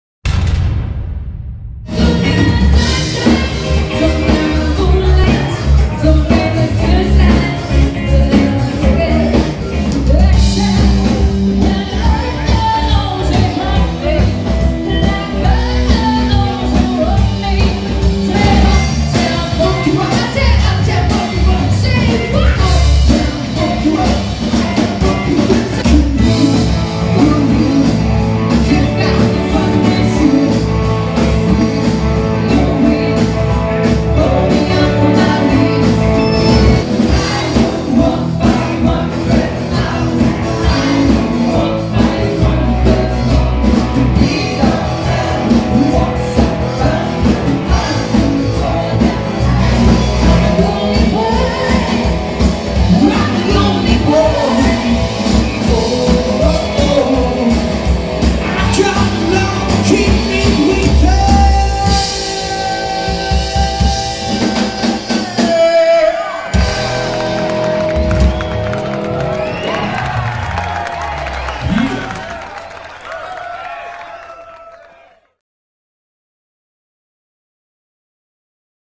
Deelish [Duo/Trio/Full band]
Deelish
is a Top 40 covers band fronted by a sexy and powerful singer, Rosie C. Rosie has fronted many top Melbourne bands and is also touring support vocalist with Normie Rowe and Doug Parkinson from 'The Go Show!'
With her awesome vocal range and stage presence, Rosie is backed by three very talented boys who stand and deliver! These guys have, at one time or another, played in many of Melbourne’s best-known cover bands which including Louie & The Party Boyz, Xcite, Mama Jam, Chunky Jam and Sexual Chocolate to name but a few.
Many years in the scene gives us the experience and knowledge to guarantee any venue a great fun-filled night catered to the clientele.